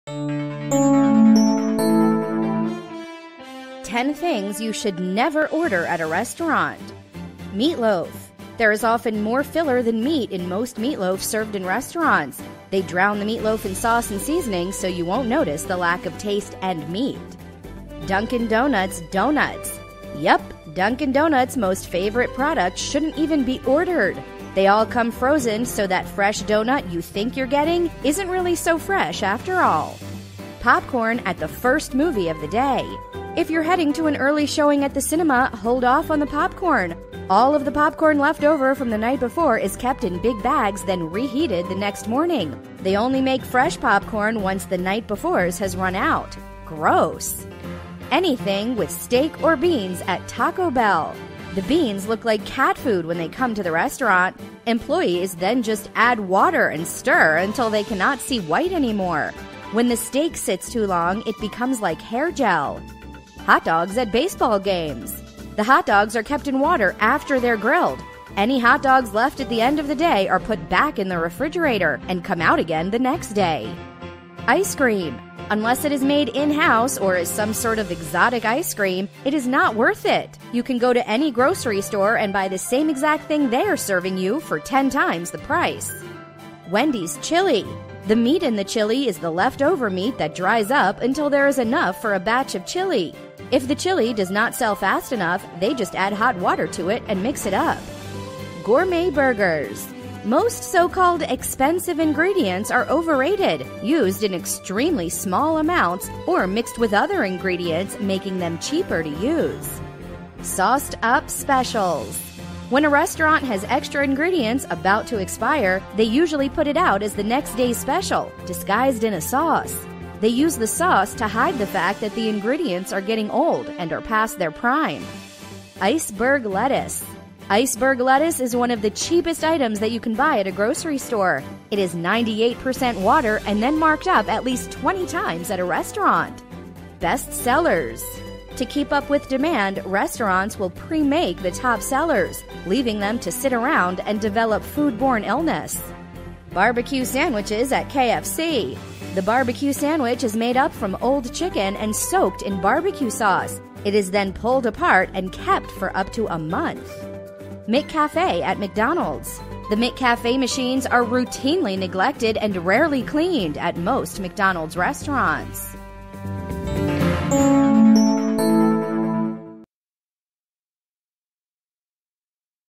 Things You Should Never Order At A Restaurant
Meatloaf

There is often more filler than meat in most meatloaf served in restaurants. They drown the meatloaf in sauce and seasonings, so you won't notice the lack of taste and meat.

Dunkin' Donuts donuts
Yep, Dunkin' Donuts' most famous product shouldn't even be ordered. They all come frozen so that fresh donut you think you're getting isn't really so fresh after all.

Popcorn at the first movie of the day
If you're heading to an early showing at the cinema, hold off on the popcorn. All of the popcorn leftover from the night before is kept in big bags then reheated the next morning. They only make fresh popcorn once the night before's has run out! Gross.

Anything with steak or beans at Taco Bell
The beans look like cat food when they come to the restaurant. Employees then just add water and stir until they cannot see white anymore. When the steak sits too long, it becomes like hair gel.

Hot Dogs at baseball games
The hot dogs are kept in water after they are grilled. Any hot dogs left at the end of the day are put back in the refrigerator, and come out again on the next day.

Ice cream
Unless it is made in house or is some sort of exotic ice cream, it is not worth it. You can go to any grocery store and buy the same exact thing they are serving you without the restaurant charging you ten times the store price.

Wendy's Chili
The meat in the chili is the leftover meat that dries up until there is enough for a batch of chili. If the chili does not sell fast enough, they just add hot water to it and mix it up.

Gourmet Burgers
Many restaurants try and use one expensive ingredient in a burger, and then label it "gourmet" with a significantly higher price tag. Most so called expensive ingredients are overrated, used in extremely small amounts, or mixed with other ingredients making them cheaper to use.

Sauced-up specials
When a restaurant has extra ingredients about to expire, they usually put it out as the next day's special, disguised in a sauce. They use the sauce to hide the fact that the ingredients are getting old, and are past their prime.

Iceberg Lettuce
Iceberg lettuce is one of the cheapest items that you can buy at a grocery store. It is ninety eight percent water and then marked up at least twenty times at a restaurant. Plus germs can hide in the cracks of the lettuce.